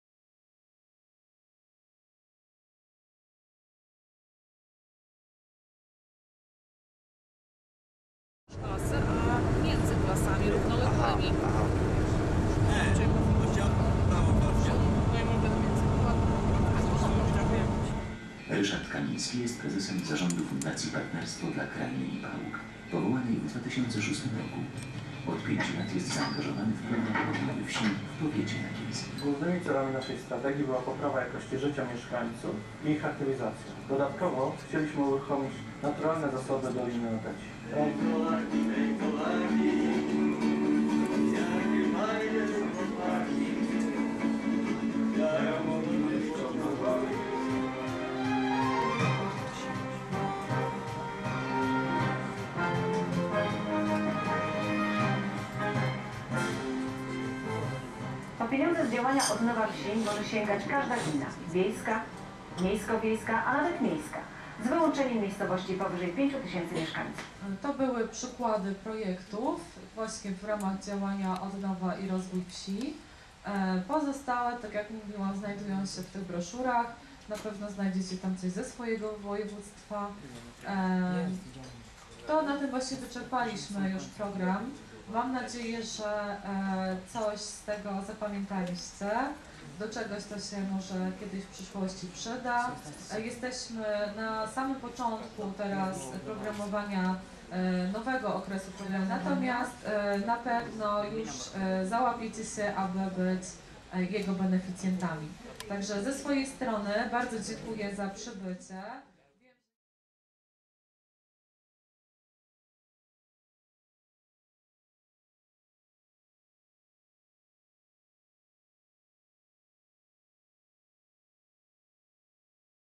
Program Rozwoju Obszarów Wiejskich
Wyjazd uczniów Zespołu Szkół Centrum Kształcenia Rolniczego w Sejnach do Warszawy na PROW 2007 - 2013.